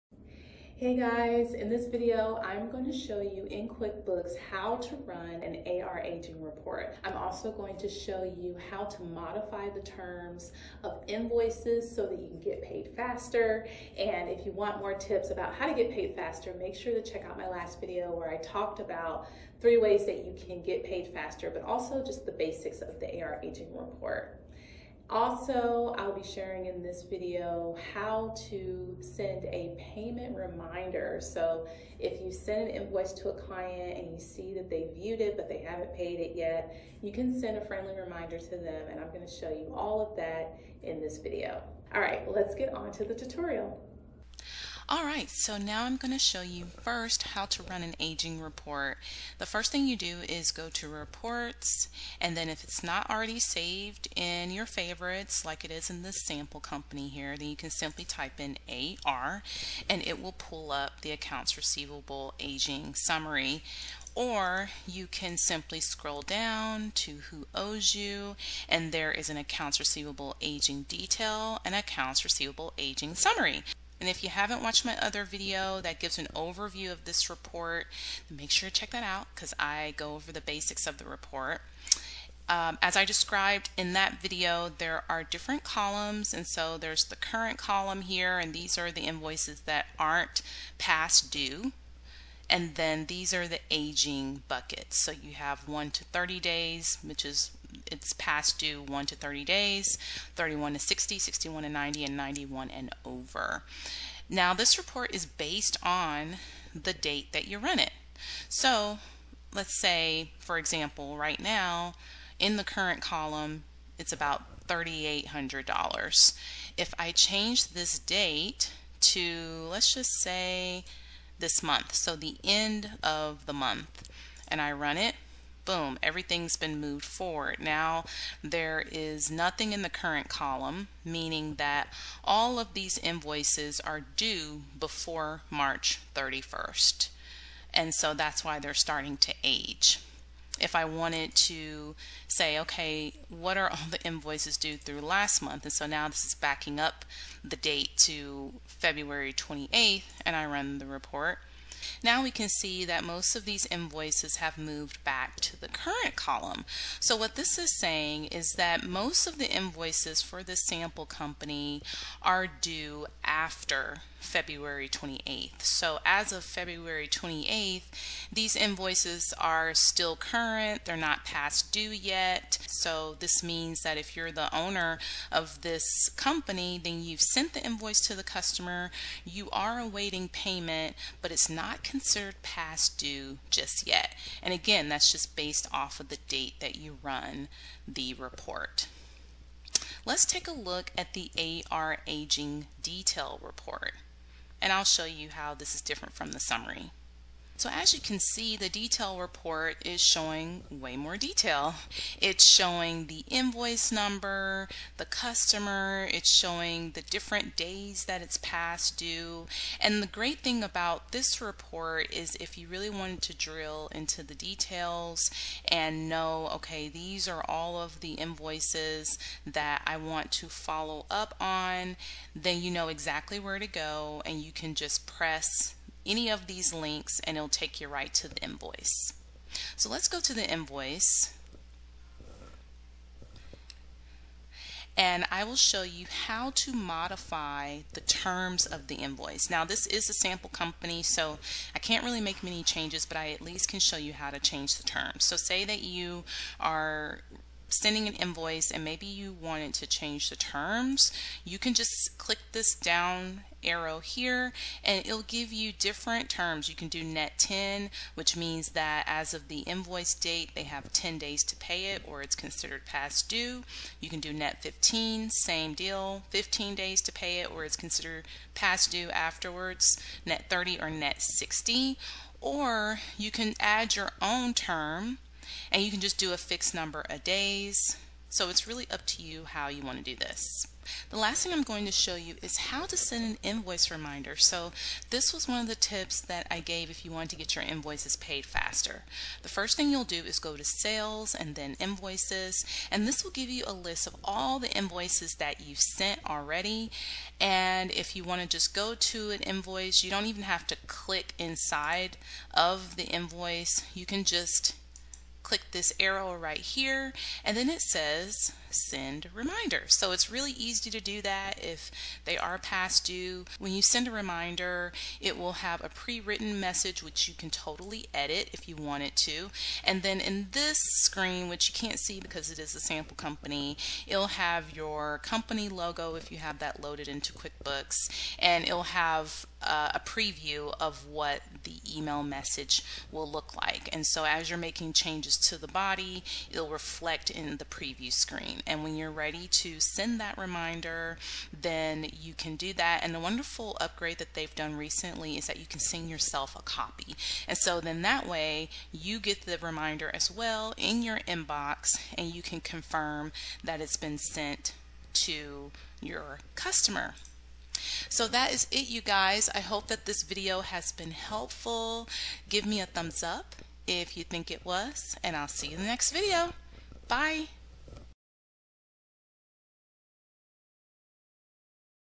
How To Run Accounts Receivable (AR) Aging Reports In QuickBooks Online 2021
⬇⬇⬇⬇ Click 'Show More' to expand ⬇⬇⬇⬇
This video shows how to run an accounts receivable aging report in Quickbooks Online (QBO). It's important to run aging reports for your business to ensure that you are getting paid in a timely manner.

I also show you:
The summary aging vs the detailed aging
How to modify the terms of the invoice
How to send an invoice payment reminder

Work with Me:
↣ QuickBooks Clean Up
↣ Tax Advisory
↣ Business Strategy Session
↣All clients must sign a client agreement.